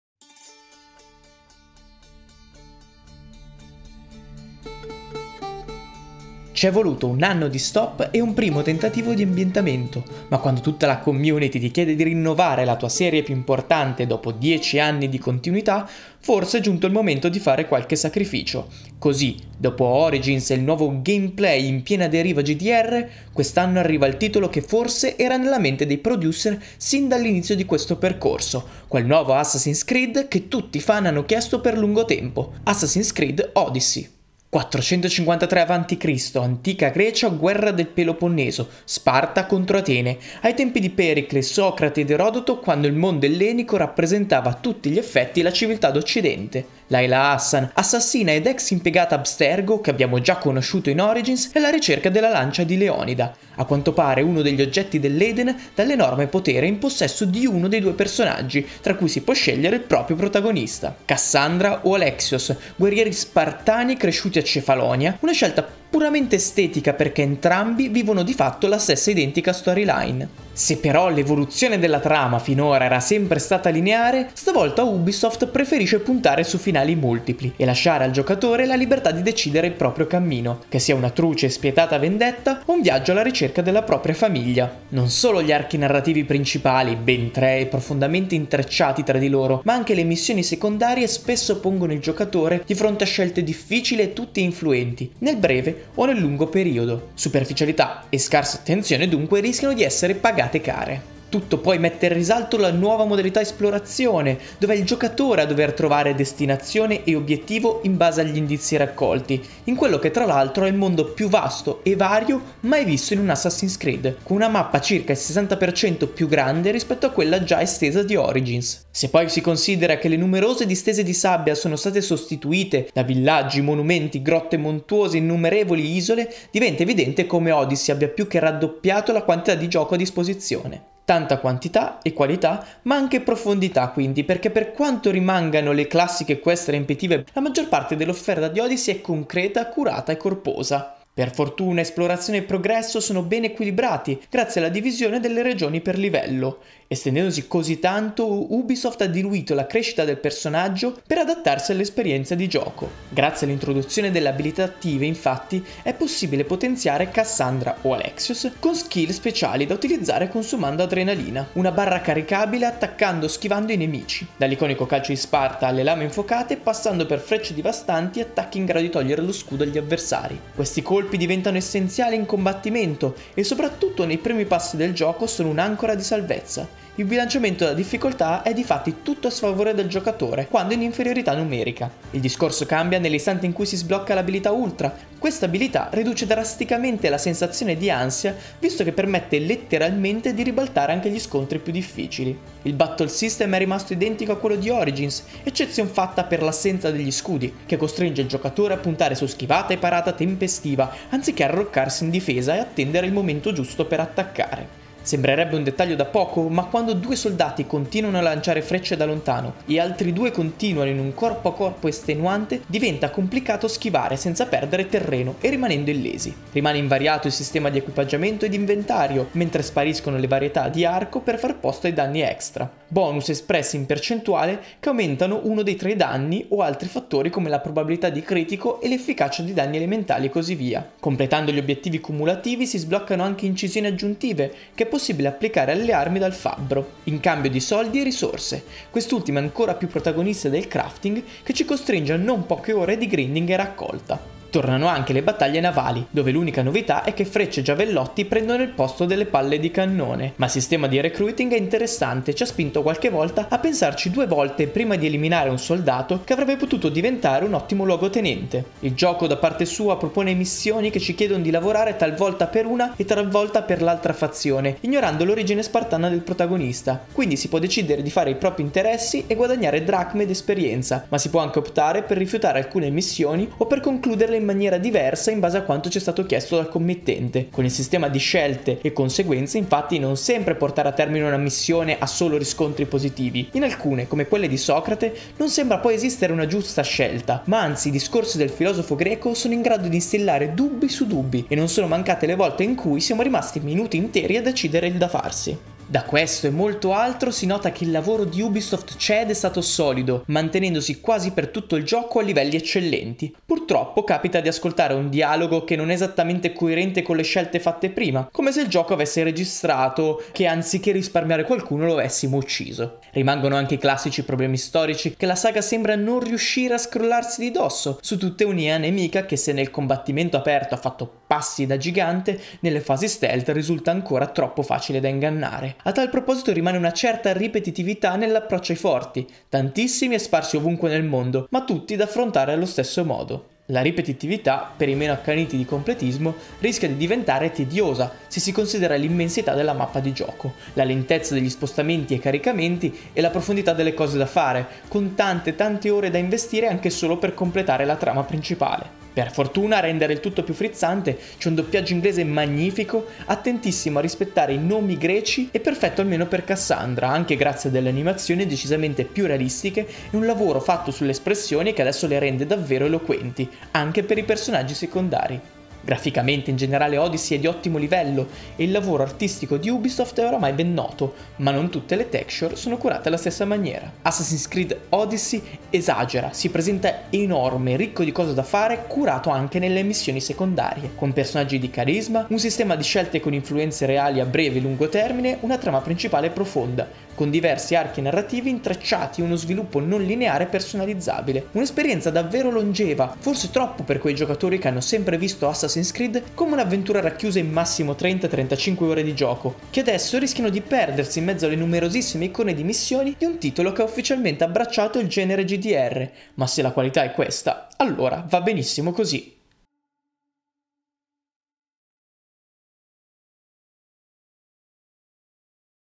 Assassin's Creed Odyssey Recensione
Assassin’s Creed Odyssey esagera, si presenta enorme, ricco di cose da fare, curato anche nelle missioni secondarie, con personaggi di carisma, un sistema di scelte con influenze reali a breve e lungo termine, una trama principale profonda, con diversi archi narrativi intrecciati e uno sviluppo non lineare, personalizzabile. Un grandissimo lavoro su animazioni, espressioni e doppiaggio inglese dà una luce diversa a tutta l’atmosfera e coinvolge il giocatore come Origins non era riuscito a fare, mentre le novità introdotte sono tutte interessanti e aggiungono varietà al gioco. Non mancano alcuni problemi storici e su tutti quello dell’IA nemica nelle fasi stealth: il mercato ci ha insegnato che si può fare molto di meglio ma non abbiamo notato passi in avanti in questo capitolo, a differenza della stessa nei combattimenti a viso aperto dove al giocatore non viene data tregua. Un’esperienza davvero longeva, forse troppo per quei giocatori che hanno sempre visto Assassin’s Creed come un’avventura racchiusa in massimo 30-35 ore di gioco, che adesso rischiano di perdersi in mezzo alle numerosissime icone di missioni di un titolo che ha ufficialmente abbracciato il genere GDR; ma se la qualità è questa, allora va benissimo così.